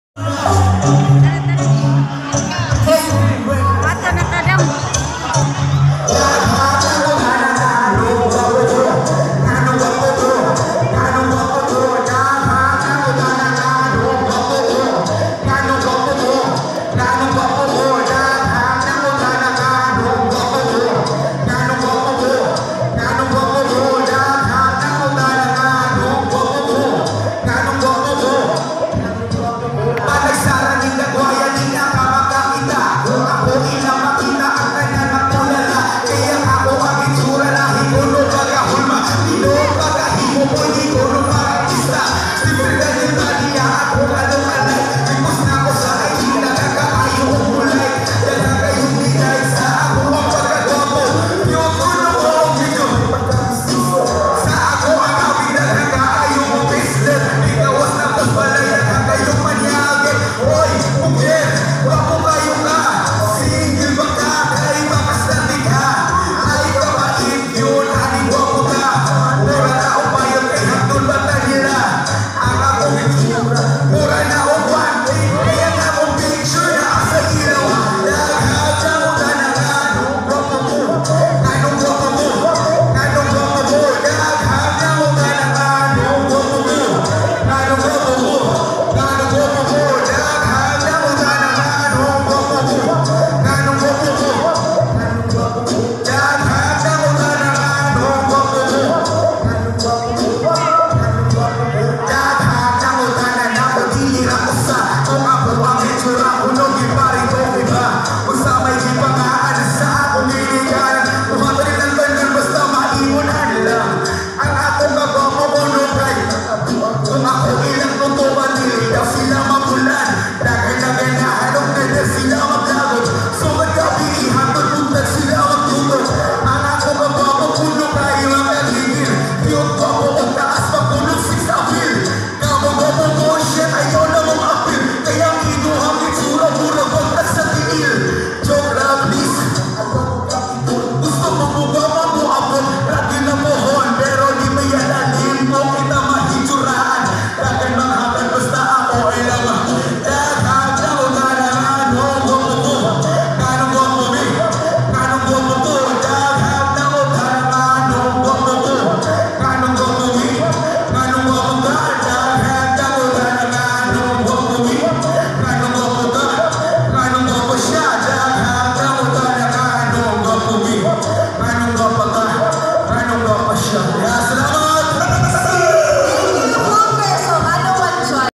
Broder Fongger Perform in Puertobello, Tudela Camotes Island Cebu...
AKP Broder  Fongger perform in Tudela Camotes Island Cebu during Broder kons Yepyep Lanusa Caravan...